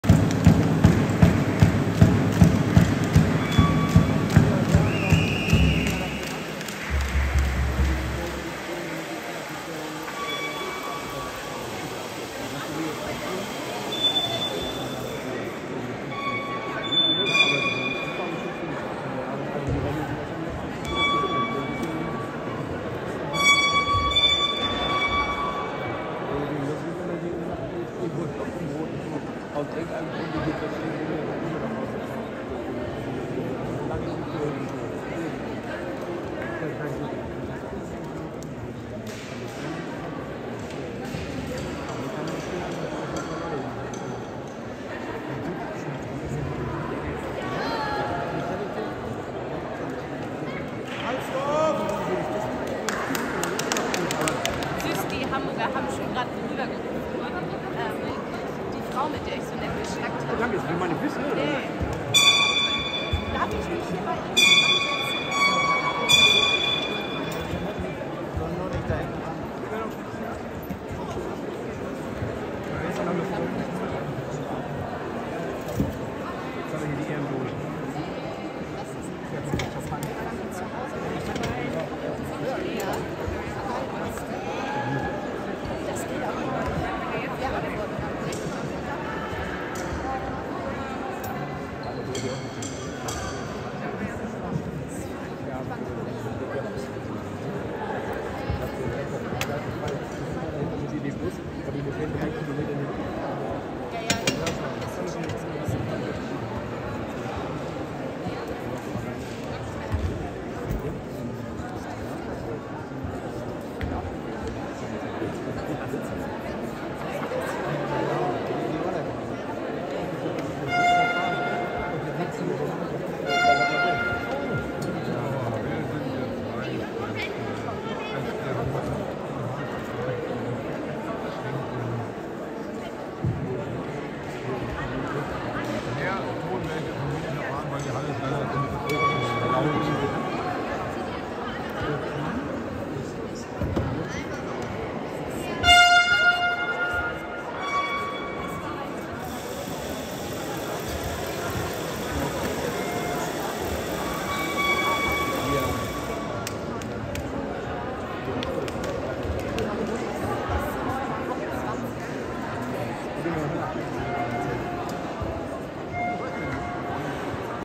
20.05.23 🤾🏼‍♀️ Gr. A Spiel 2 Begrüßung
🤾🏼‍♀️ wC Cup der Landesmeister
Gruppe A
SG Hamburg Nord vs. HSG Saalfeld/Könit